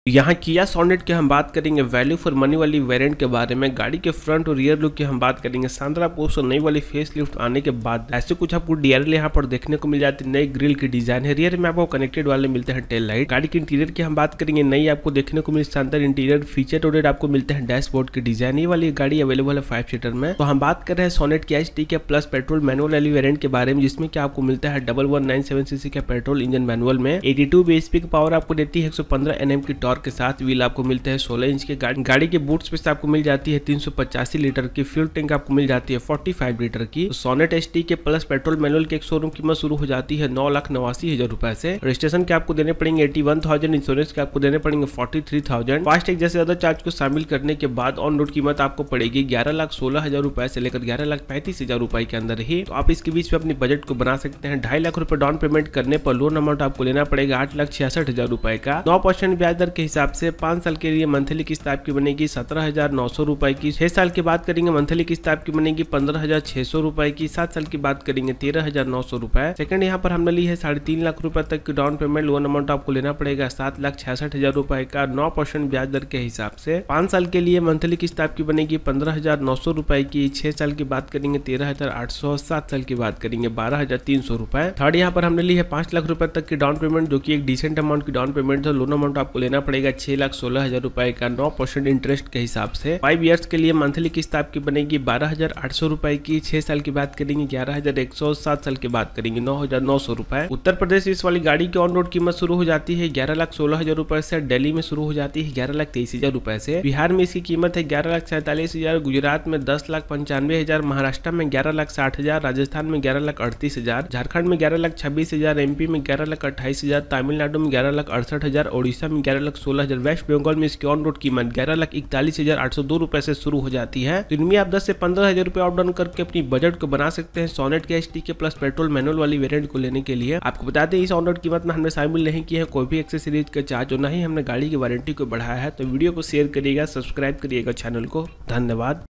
2024 Kia Sonet Petrol Price | Kia Sonet HTK Plus Manual Onroad Price 2024, Finance, Loan Price, Emi
2024 Kia Sonet HTK Plus Onroad Price. Here is Kia Sonet HTK Plus 1.2 Petrol Manual Price With 3 downpayment and loan details with monthly emi scheme.

Plan to buy 5 seater car checkout this kia sonet htk plus price with finance plan in hindi.